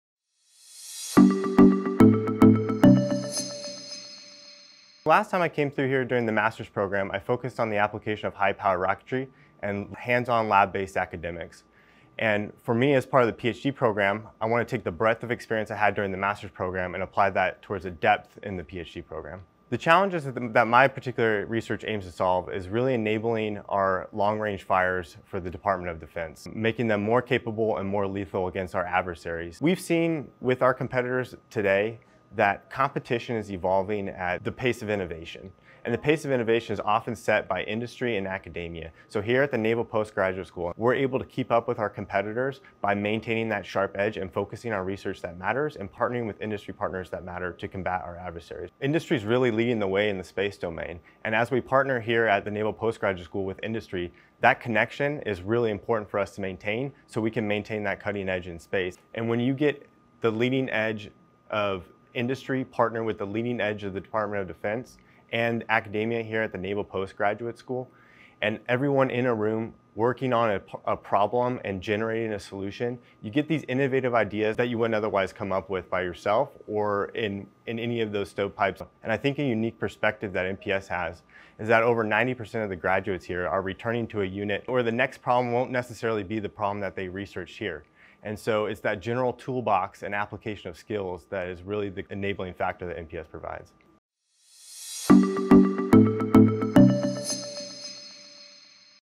Capt. Dillon Pierce on the value of space-related research & education at NPS
CAPT. DILLON PIERCE, USMC, MS in Space Systems Operations '19, PhD Candidate - Space Systems Engineering, on how the Space Systems and Operations research and education at the Naval Postgraduate School develops capable leaders and accelerates innovative solution development.